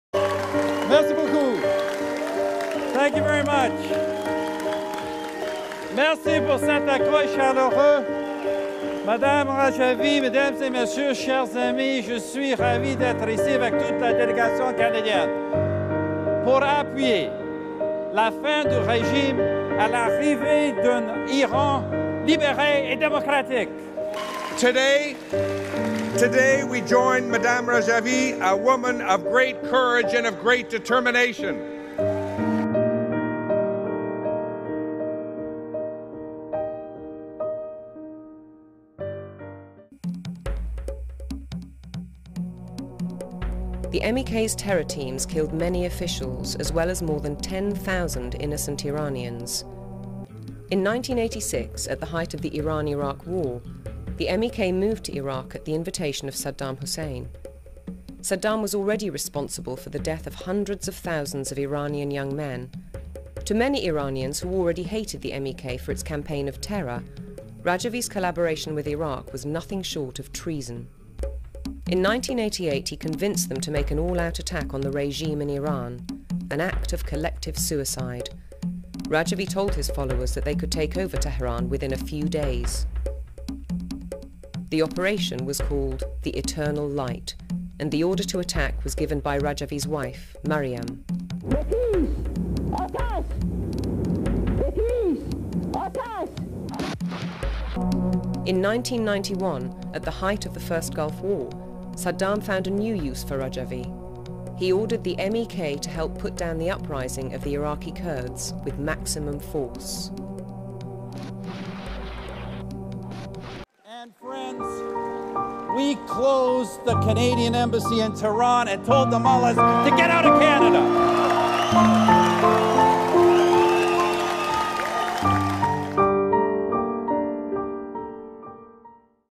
Stephen Harper, John Baird, MP Candice Bergen and MP Judy Sgro Support Mujahedin-E-Khalq (MEK/NCRI)
Former prime minister Stephen Harper, former foreign affairs minister John Baird, current Members of Parliament Hon. Candice Bergen, MP, MP Judy Sgro attended Mujahedin-E-Khalq's gathering in Paris and spoke in support of the MEK. MEK/NCRI is a radical cult that Harper’s own government considered a terrorist organization as recently as 2012.

It's not clear whether other speakers at the conference were paid. However, The NCRI/MEK have been known for paying high speaking fees.

Harper, Baird, MP Bergen and MP Sgro must explain for Canadians why they support an extreme cult with radical views and a long history of violence and terror.

MEK has no support among Iranian people or among Iranian-Canadians. Guardian reported few days ago from the conference that "around half of the attendees were Iranian. The other half consisted of an assortment of bored-looking Poles, Czechs, Slovakians, Germans and Syrians who responded to a Facebook campaign promising travel, food and accommodation to Paris for a mere €25. Hundreds of Syrian refugees settled in Germany also attended. Many snoozed under trees during speeches."